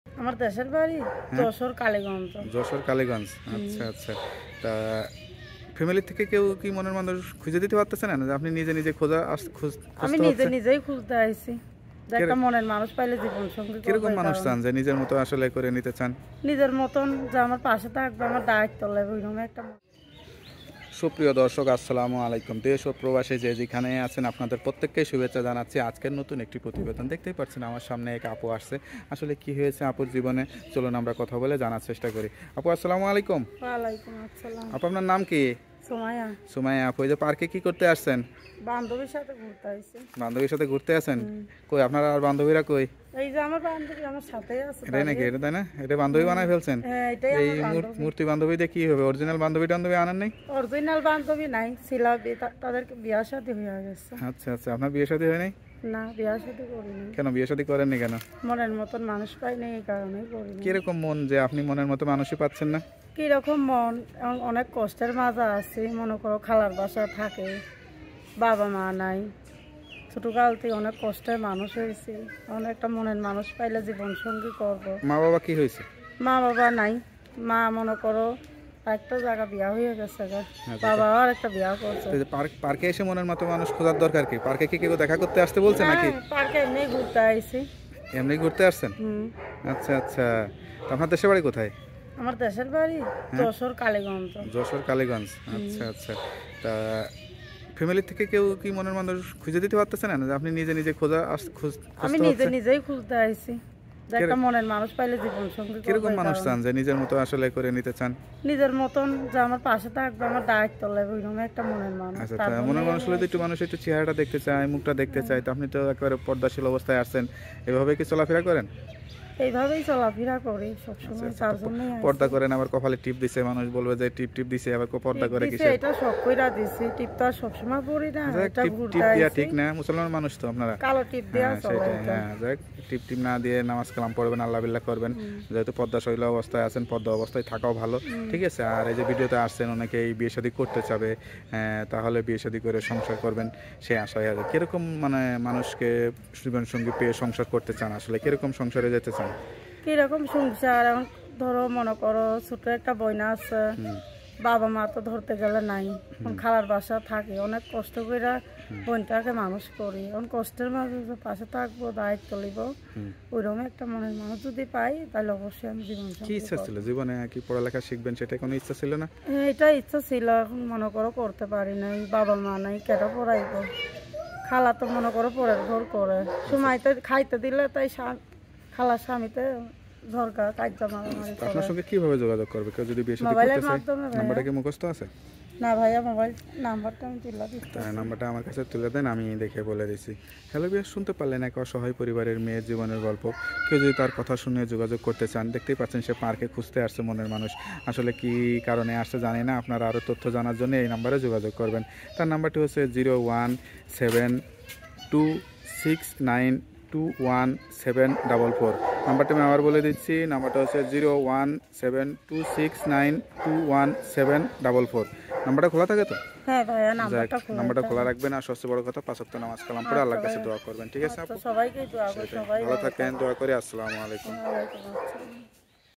আমার গ্রামের বাড়ি যশোর কালিগঞ্জ এখন নিরুপায় হয়ে নিজের বিয়ের জন্য নিজেই ছেলে খুঁজতে আসছি।
আমার গ্রামের বাড়ি যশোর কালিগঞ্জ এখন নিরুপায় হয়ে নিজের বিয়ের জন্য নিজেই ছেলে খুঁজতে আসছি। জীবনের বাস্তব গল্প সত্য ঘটনা বিস্তারিত জানতে পুরো ভিডিওটি দেখুন। কেউ যদি তার সঙ্গে যোগাযোগ করতে চান বা বিয়ে করতে চান তাহলে তার মোবাইল নাম্বারটি ভিডিওর মধ্যে দেওয়া আছে।ওই নাম্বারে যোগাযোগ করুন ধন্যবাদ।